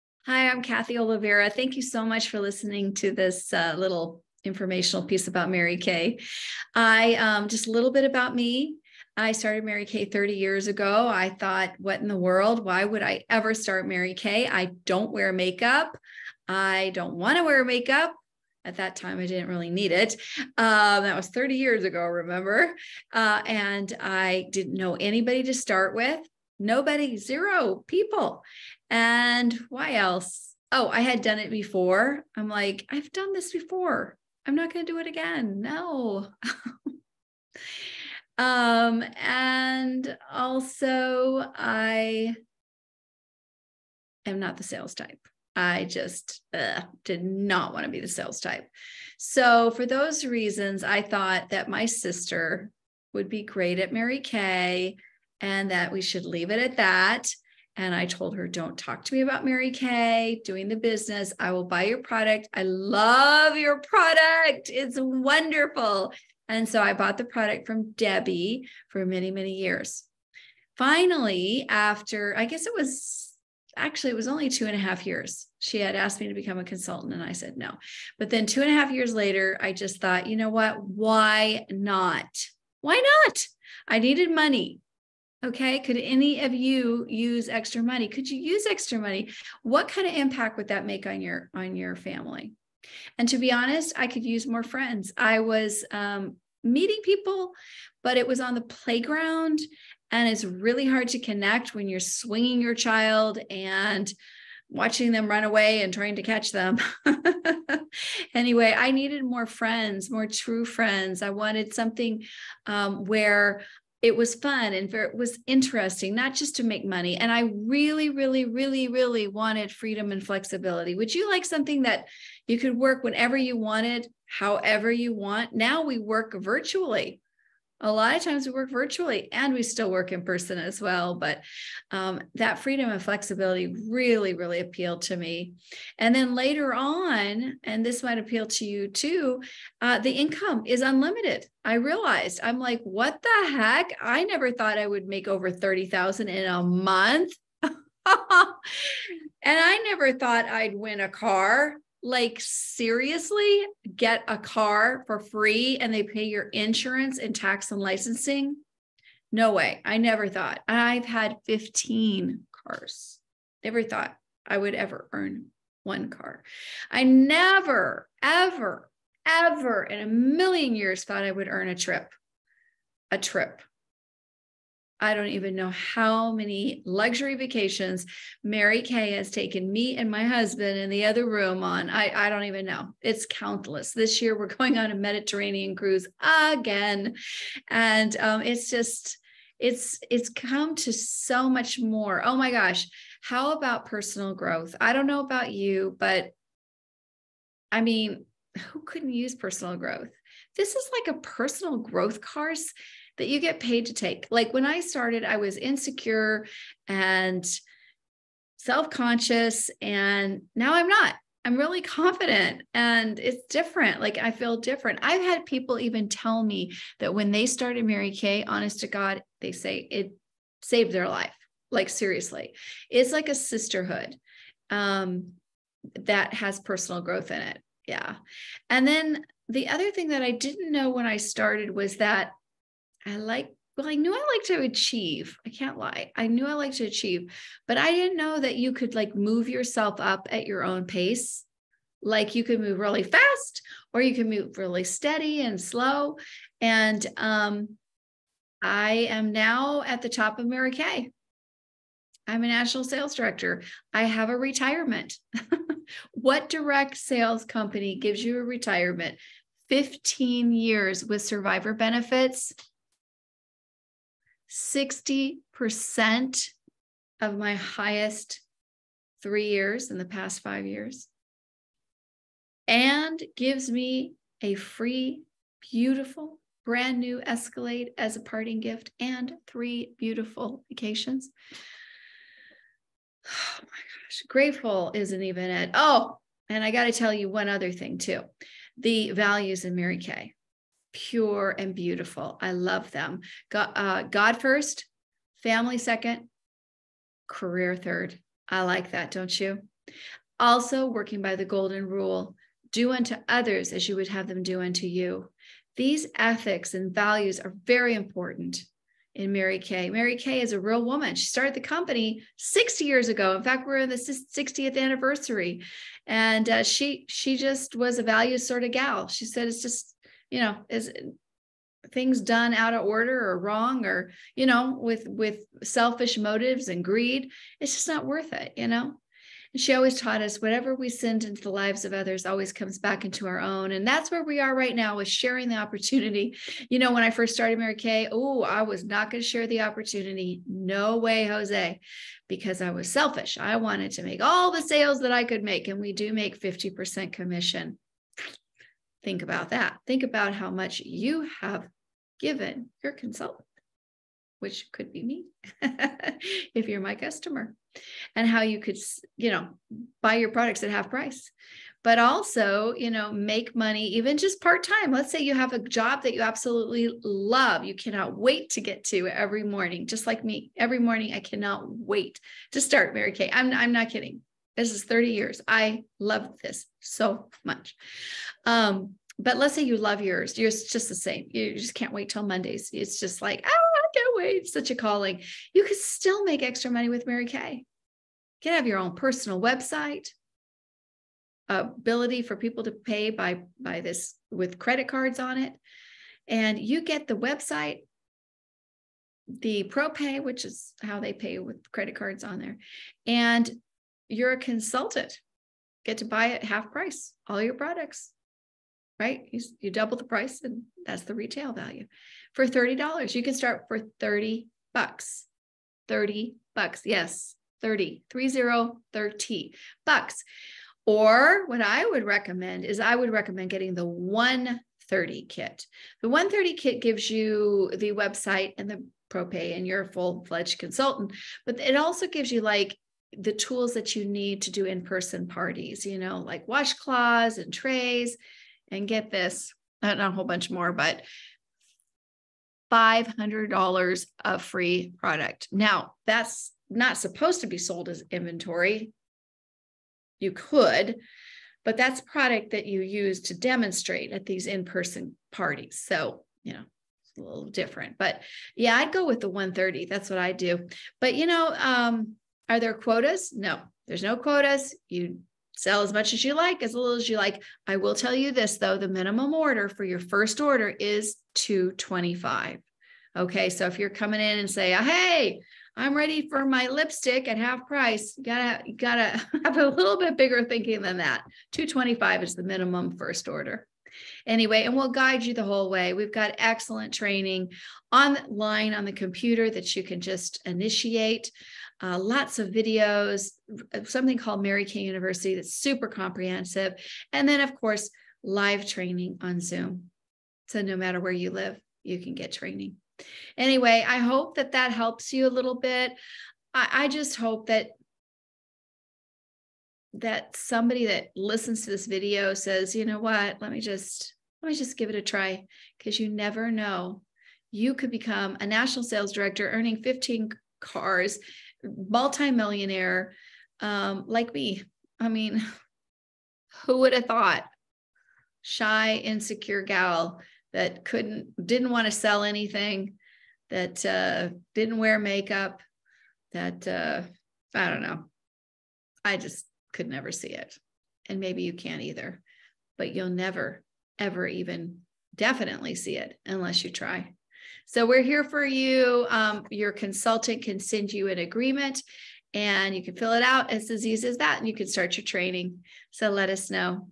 My Mary Kay Journey
Why not give Mary Kay a try?  So much to gain besides extra money.  Freedom and flexibility.  Positive friends. Personal growth. Tax write-offs.  Free cars. High integrity and values in the company.  Unlimited income and upward mobility.  Trips around the world.  And more!!